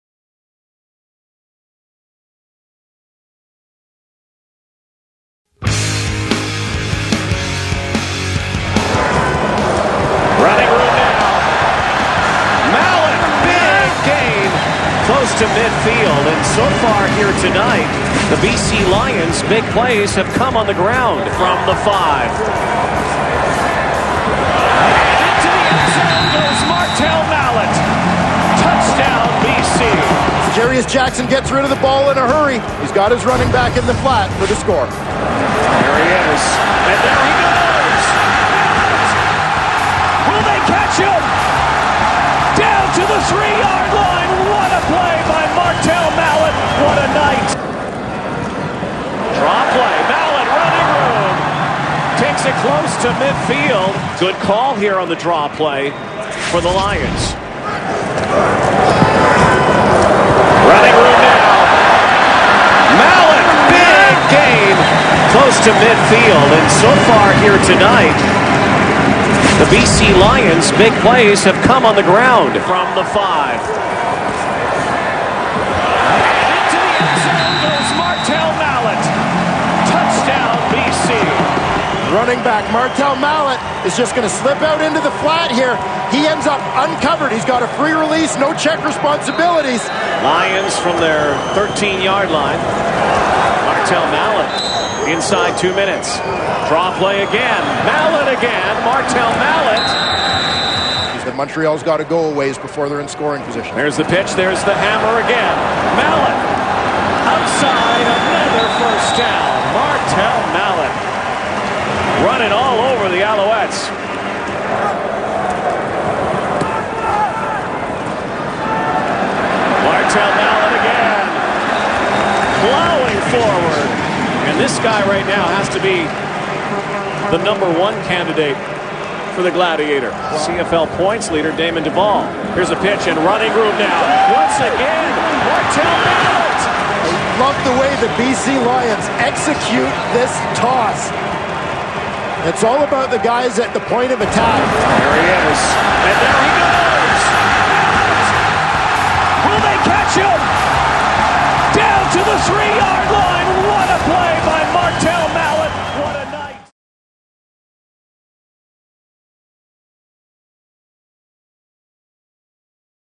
Martell "Hammer" Mallett Rushes to set BC Lions Record.
CFL.ca Staff.

VANCOUVER -- In the wake of a wild 19-12 victory over the Montreal Alouettes at BC Place on Friday night, the Lions had more than just a victory to celebrate as running back Martell Mallett established a new team record for rushing yards in a game with 213.

Rookie running back Martell Mallet enjoyed a dominating performance with a total of 21 carries for 213 yards, breaking Sean Millingtons 212-yard record set in 1997 versus the Saskatchewan Roughriders.  It was a night of firsts for the Arkansas Pine-Bluff grad who rushed for more than 100 yards for the first time in his CFL career and also hauled in a touchdown pass late in the second quarter to draw the Lions even at seven.